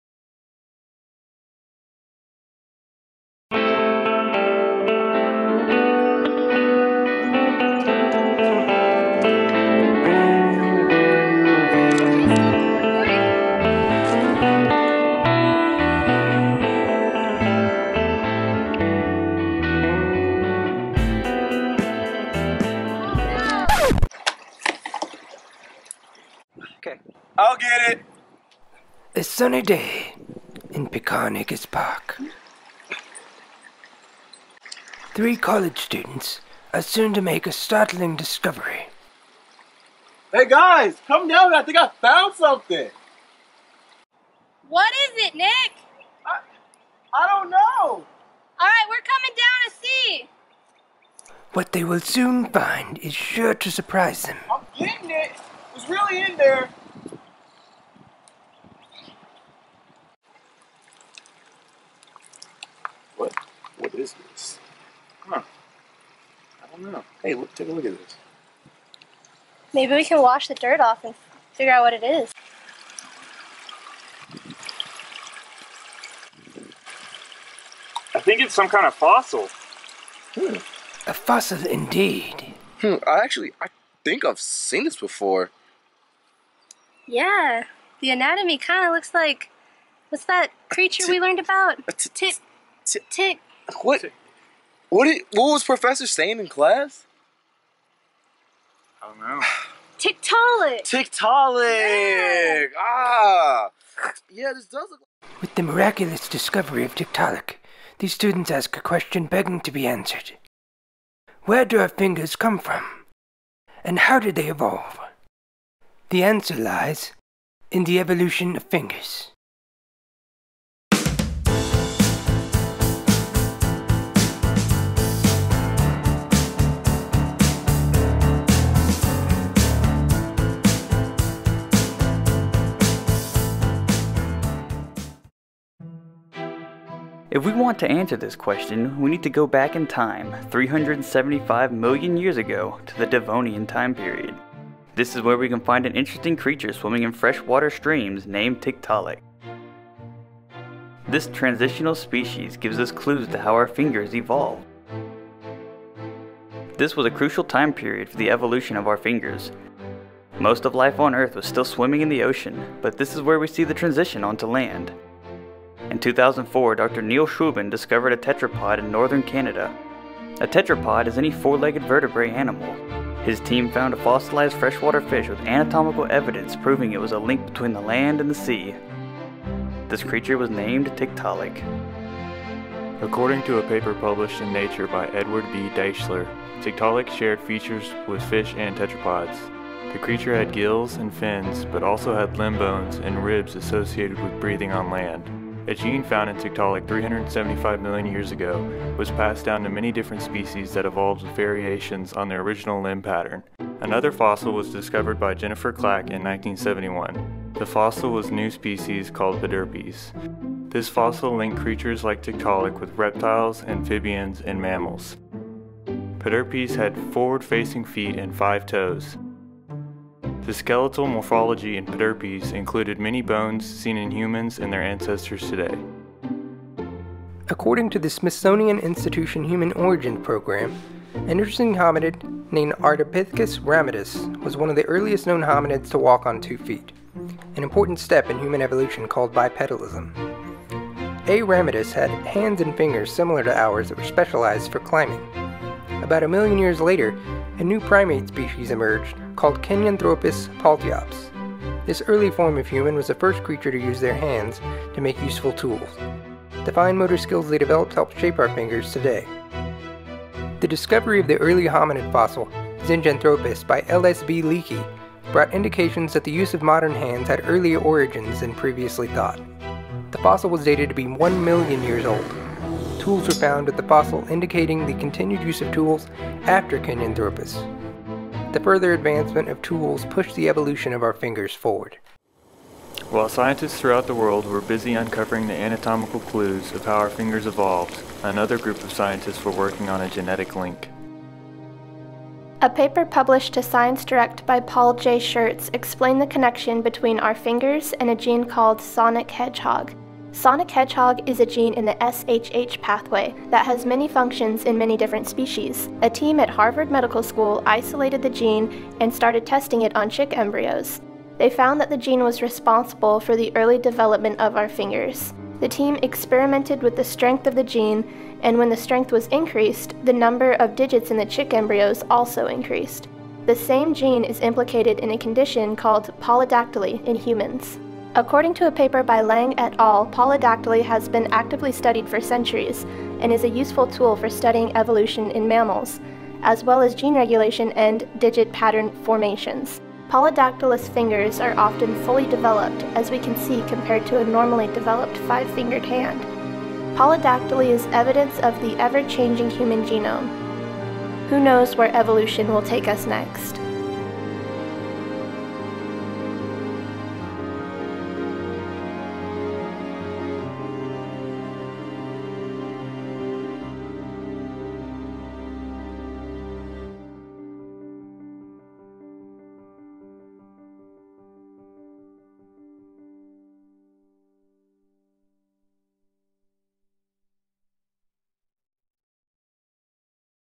The Evolution of Fingers
A concise evolution of human fingers from early development in Tiktaalik to the present day.